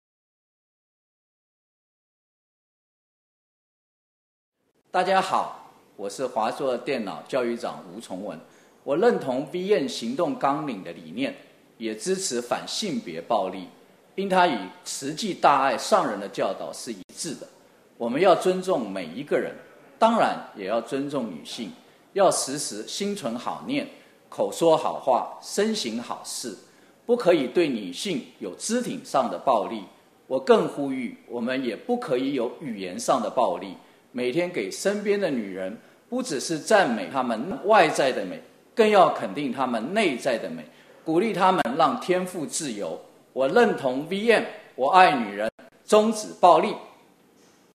【 V-MEN 】華碩電腦教育長-吳崇文
感謝華碩電腦教育長-吳崇文為勵馨拍攝示範影片~邀請大家一同認同V-MAN，一起愛女人，終止暴力!!